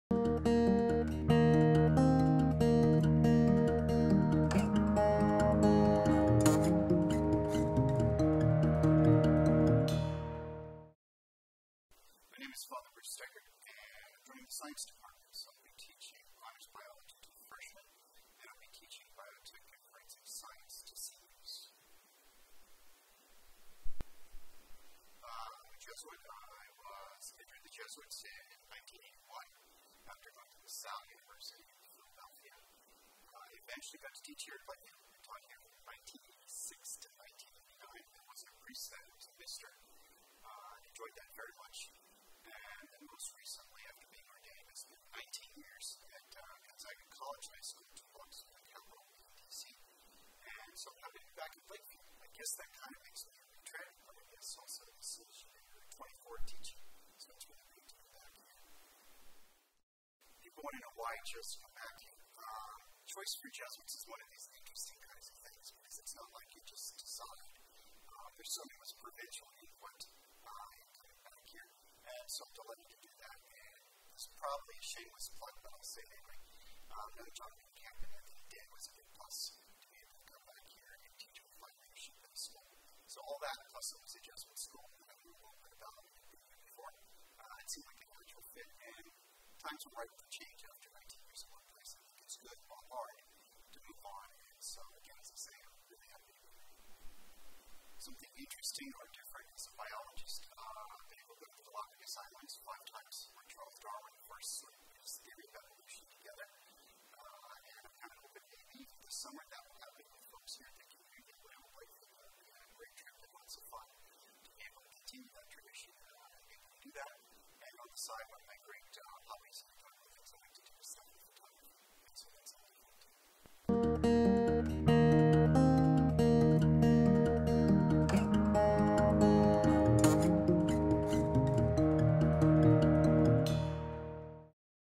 ew Teachers @ Blakefield: Fr. Bruce Steggert, S.J.
Meet Loyola's new science teacher, Fr. Bruce Steggert, S.J.